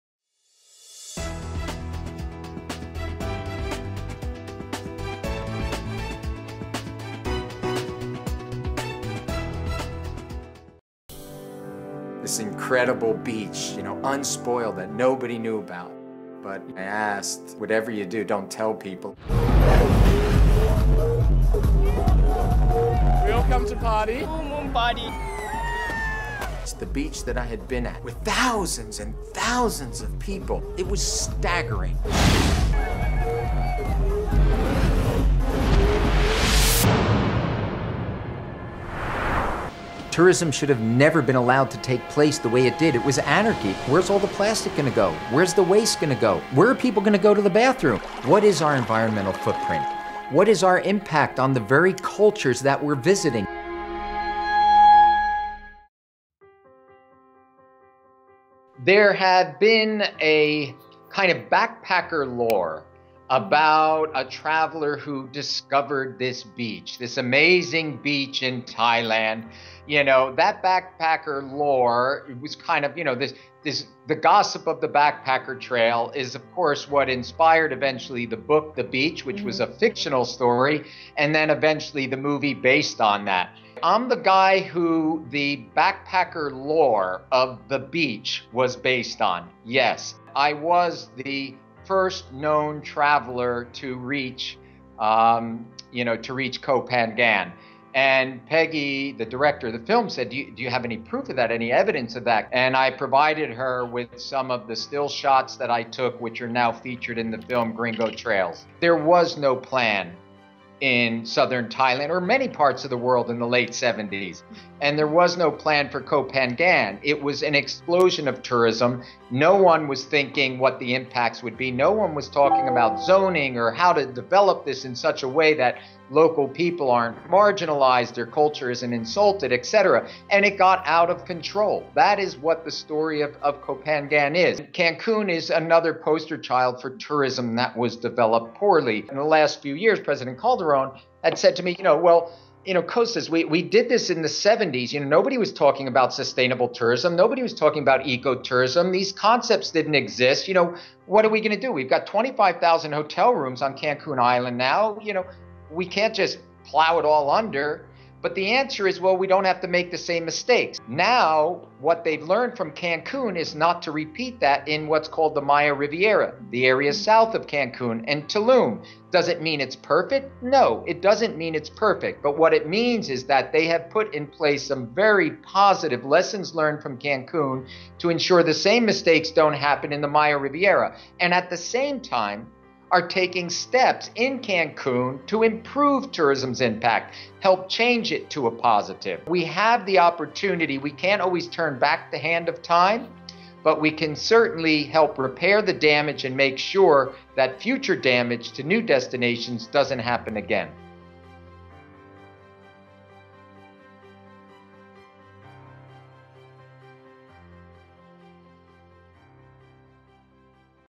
What Happens When Tourists Discover Paradise?
The story of Koh Phangan, Thailand is an all-too-familiar one in the tourism industry: discovered by backpackers, embraced by adventurers, and devastated by mass tourism.

Discover how the Full Moon Party destroyed this island paradise and what we can learn from these hard lessons.